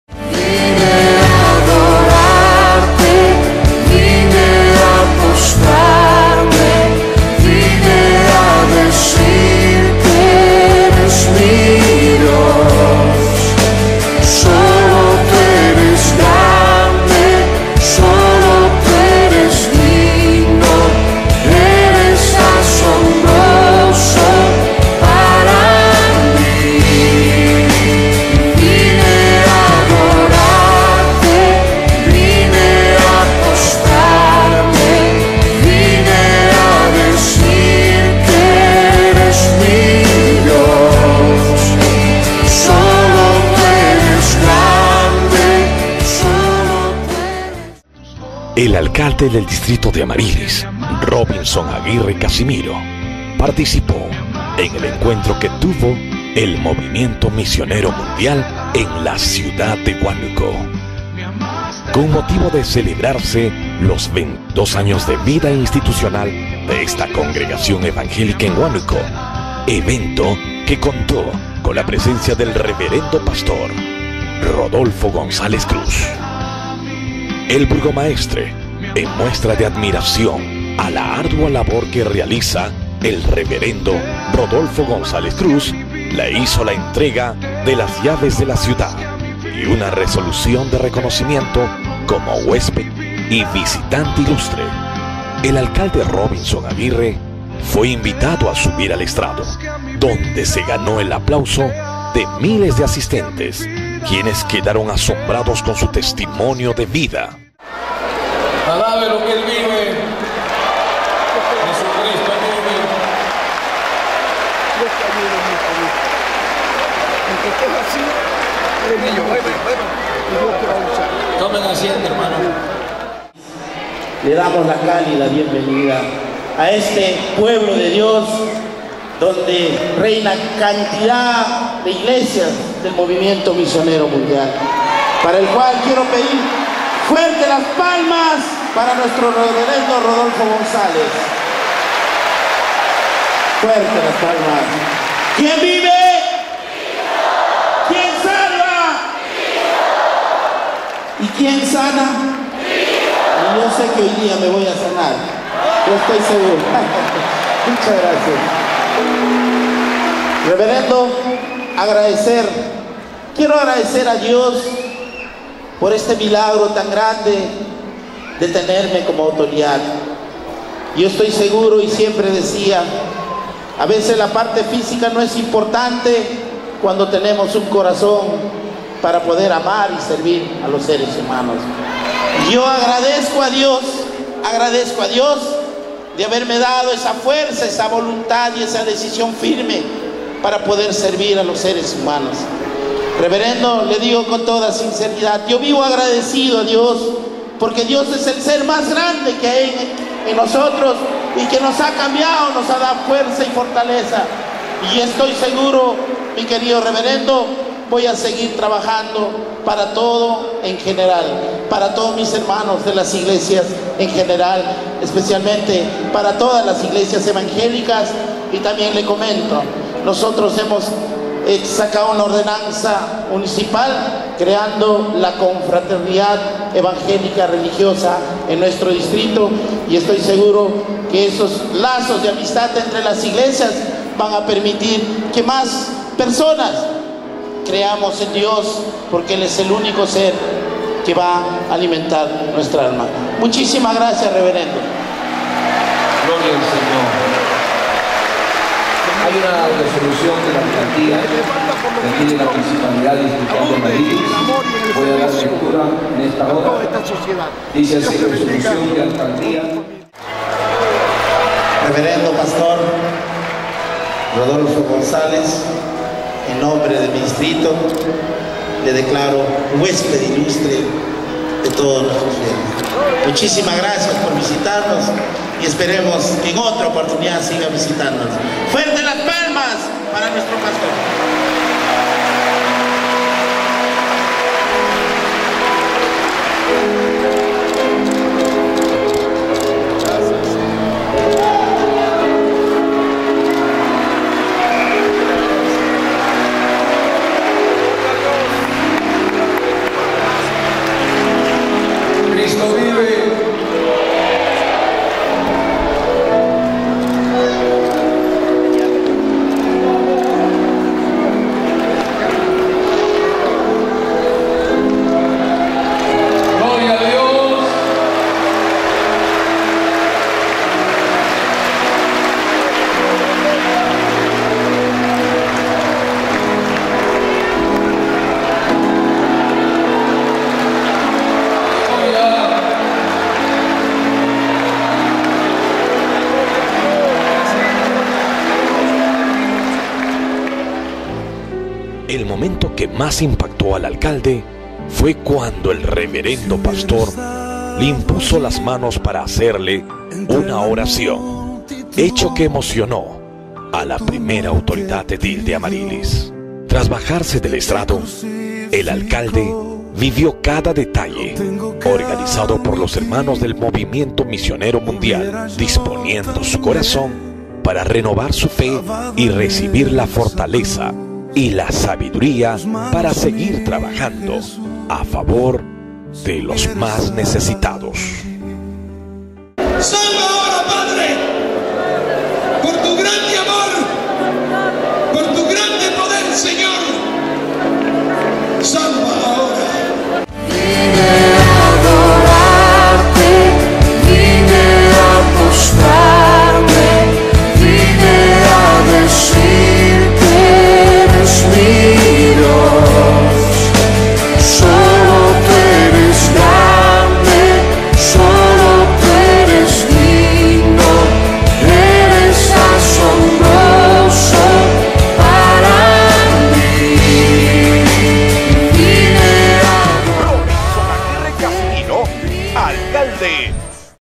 REVERENDO Rodolfo Gonzáles Cruz en AMARILIS
ALCALDE DE AMARILIS PARTICIPÓ EN CONGREGACIÓN ENCUENTRO DEL MOVIMIENTO MISIONERO MUNDIAL